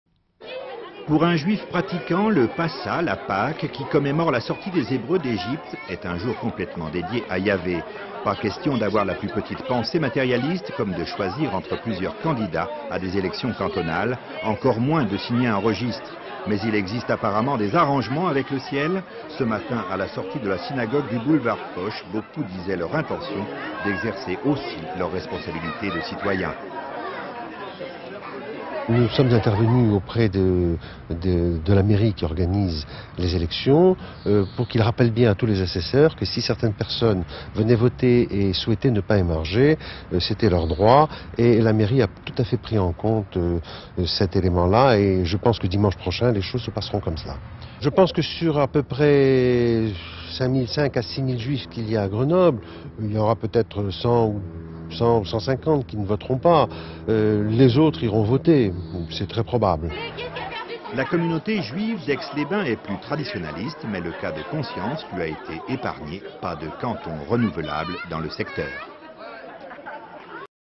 La communauté juive et le vote à Grenoble
27 mars 1994
Pour la PAQUE juive aucune activité politique ne devrait venir troubler les fidèles pourtant beaucoup ont accompli le devoir électoral Images d'archive INA